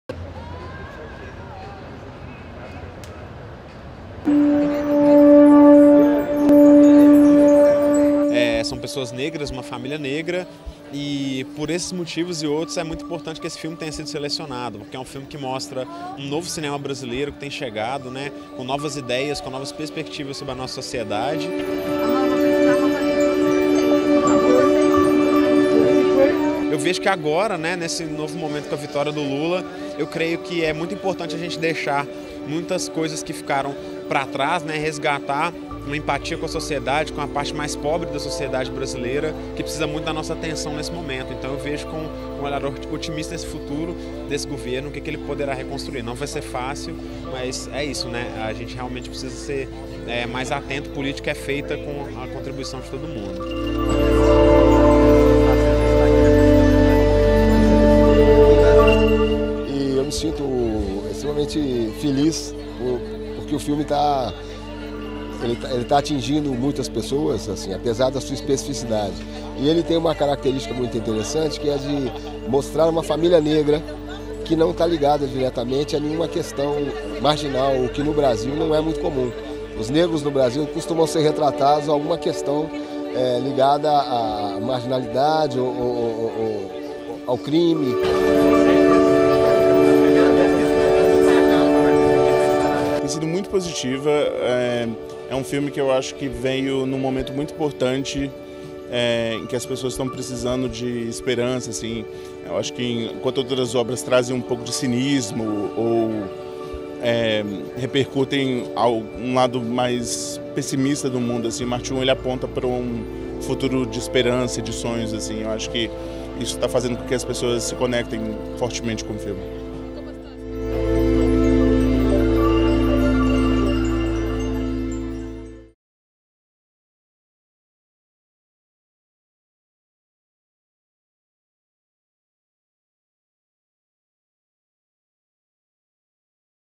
Elenco do candidato ao Oscar "Marte Um" caminha no tapete vermelho em Los Angeles
O elenco e a equipe técnica de “Mars One”, filme brasileiro indicado ao Oscar de Melhor Longa-Metragem Internacional, caminham pelo tapete vermelho da noite de abertura do Hollywood Brazilian Film Festival, em Los Angeles.

É a primeira vez que o Brasil inscreve um filme feito por um diretor negro. “Mars One” ilustra “um novo cinema brasileiro, com novas ideias e novas perspectivas sobre a nossa sociedade”, diz o diretor Gabriel Martins.

Nike proíbe nomes como 'Exu', 'Ogum' e 'Maomé' da camisa da seleção, mas libera referências cristã

Thumbnail:
Paul Archuleta / GETTY IMAGES NORTH AMERICA / Getty Images via AFP

Siga no Instagram e no Facebook: @almapretajornalismo
Siga no Twitter: @alma_preta